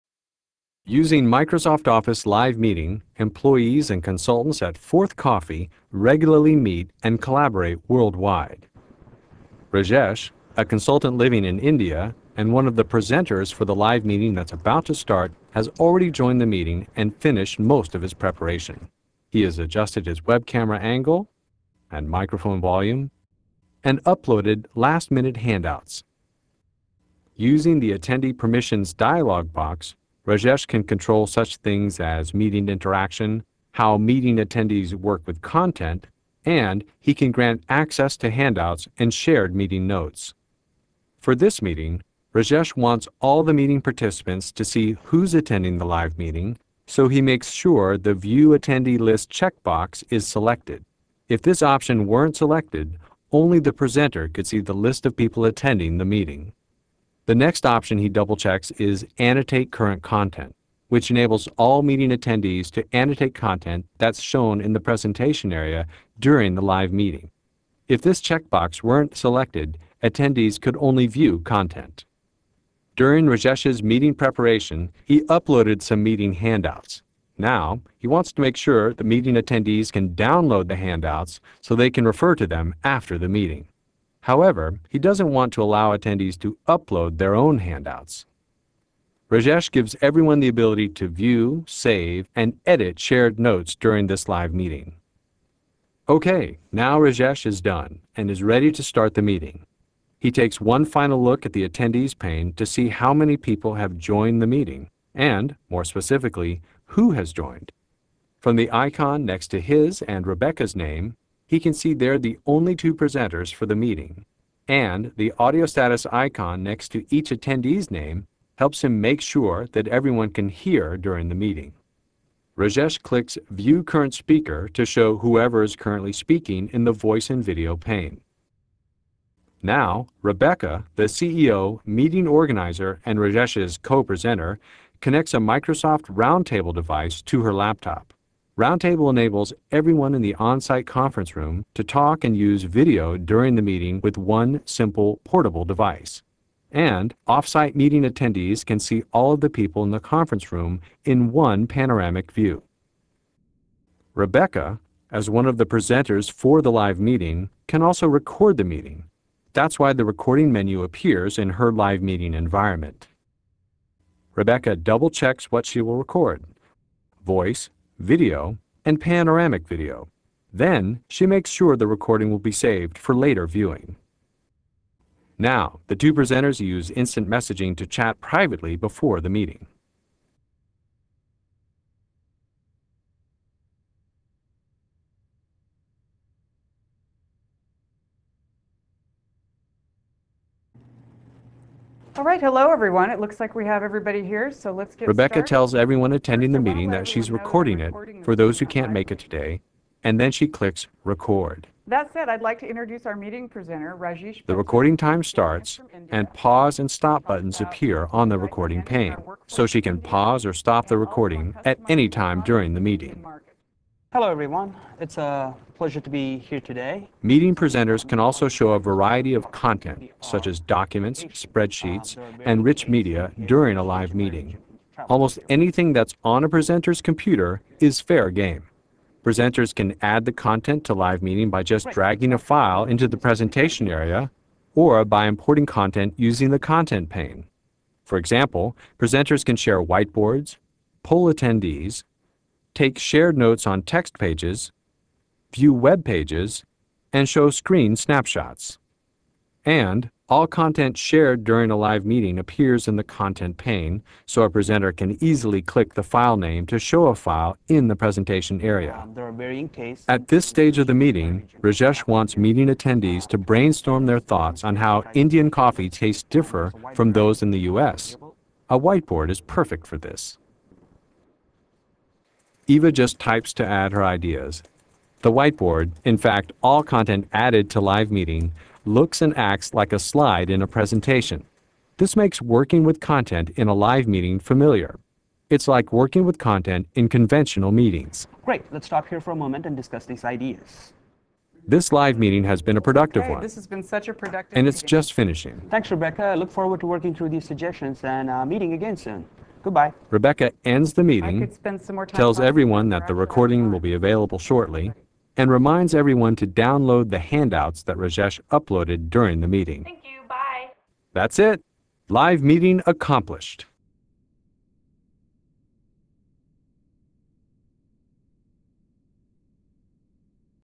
Office Live Meeting Demo 3: Get down to meeting details
Microsoft Office Live Meeting is a conferencing solution for online meetings, training, and events. Learn how you can schedule, join, or present a Live Meeting using features like audio, video conferencing, and uploading handouts. In this demo, learn about meeting permissions and how to record a meeting. Get an overview of meeting content options, see examples of meeting interactions, and meet Microsoft RoundTable.